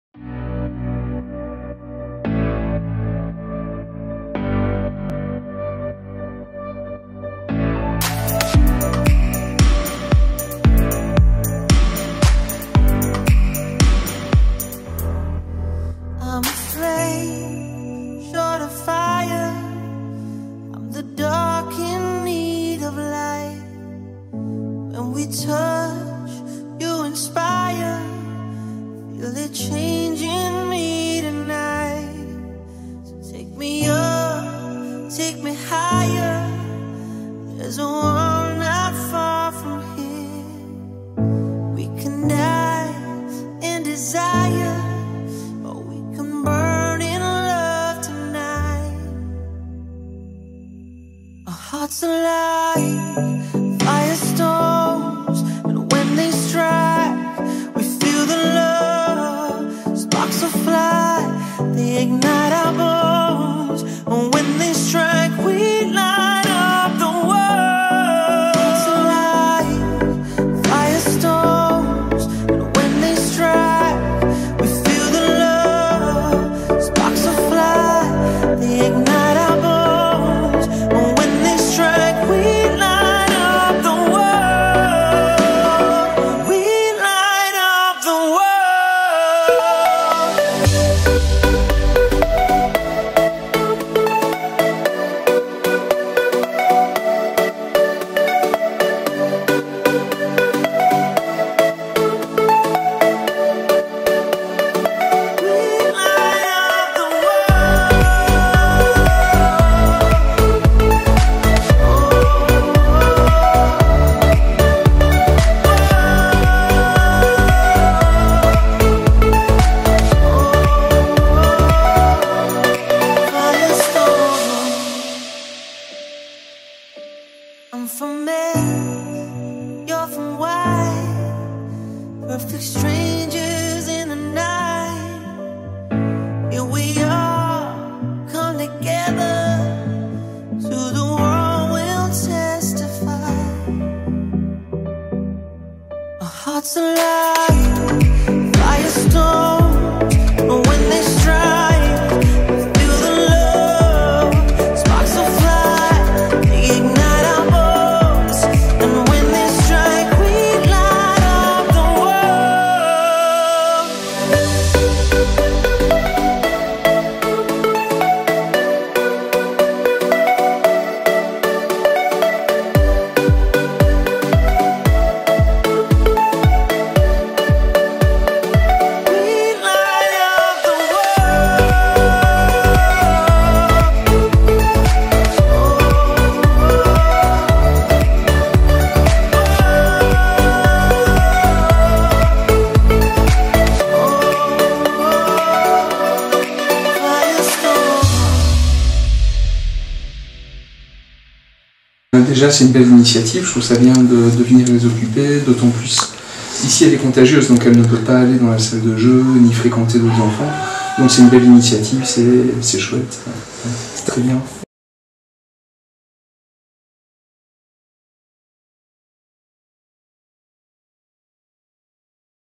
Hôpiclown avec Totoche et Maya
Ce mercredi 22 avril j'ai été filmer Totoche et Maya à l'hôpital de Dinant pour l'association d'Hôpiclown